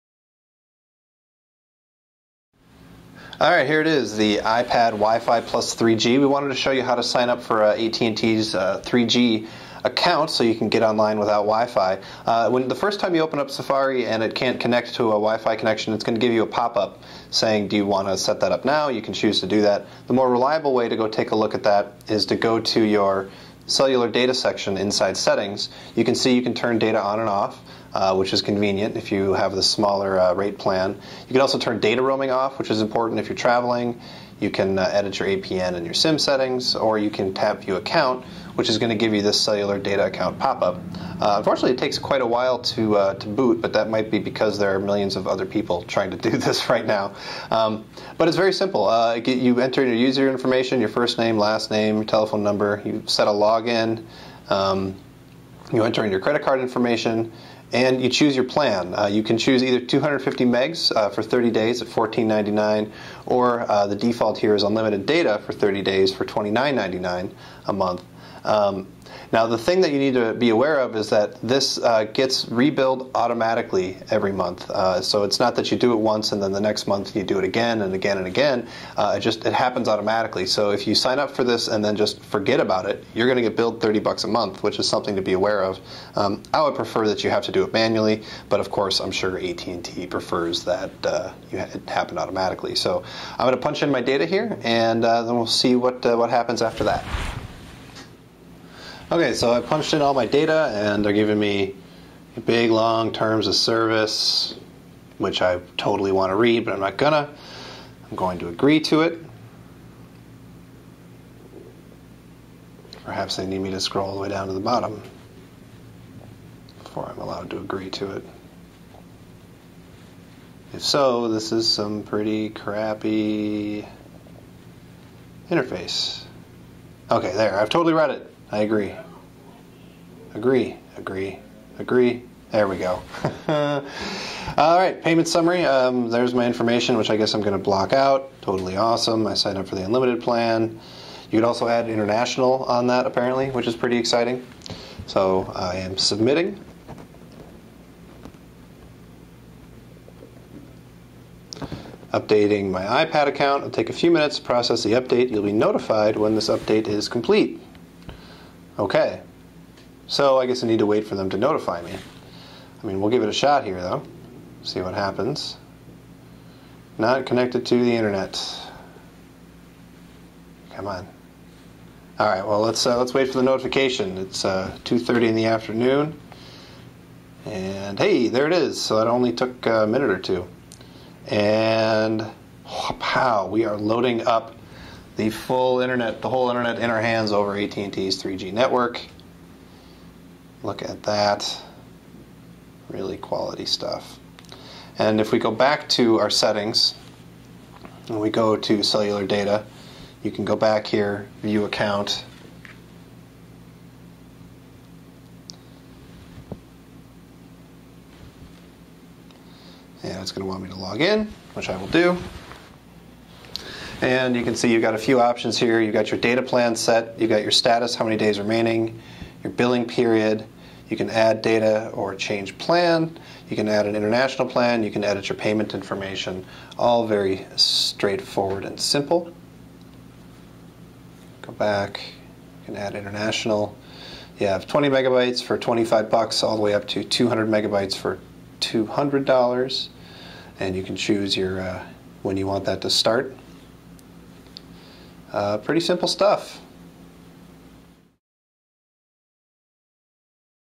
iPad 3G: Setting up your AT&T Account
here's how to set up your AT&T account so you can get data on your iPad WiFi + 3G